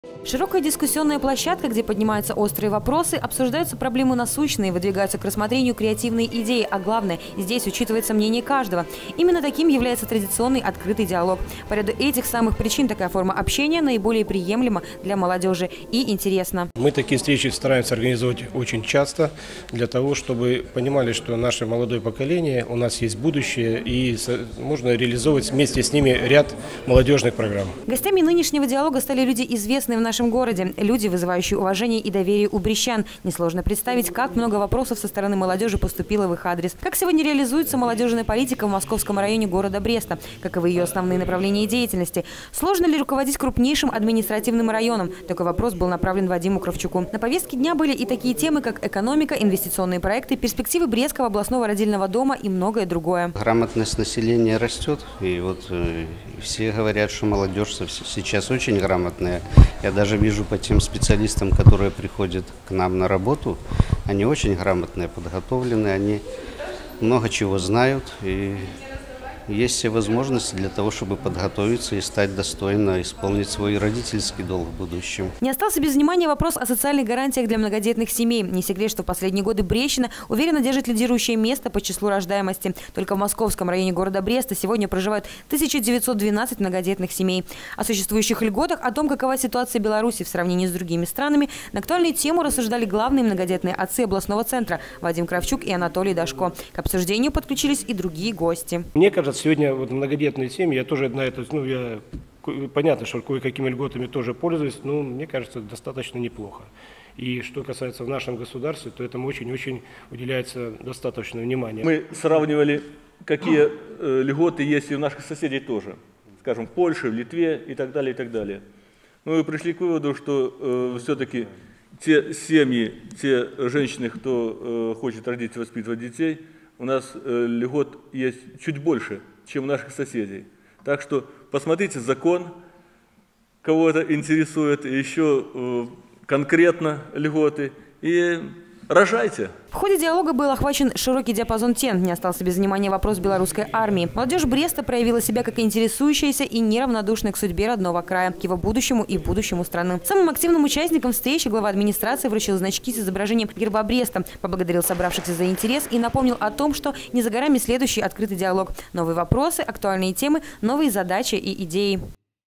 2016-08-23 г. Брест. Открытый диалог «Беларусь – государство для молодежи». Новости на Буг-ТВ.
Журналист С. Бережная Оператор Д. Величкевич
«Беларусь - государство для молодежи» - под таким лозунгом прошло традиционное мероприятие - открытый диалог брестских подростков с представителями органов власти, здравоохранения, общественными деятелями. Дебаты такого рода – отличная возможность для юношей и девушек задать волнующие вопросы, обсудить злободневные темы, высказать свои взгляды и предложения в отношении тех или иных проблем.
Молодые люди не упустили возможности поучаствовать в нынешнем диалоге, тем, более с такими гостями - руководством Московского района Бреста и депутатами Палаты представителей Национального собрания Республики Беларусь. Вопросов накопилось немало. О том, что волнует студенческую молодёжь Бреста, какие актуальные темы обсудили в ходе встречи, узнавала наш корреспондент.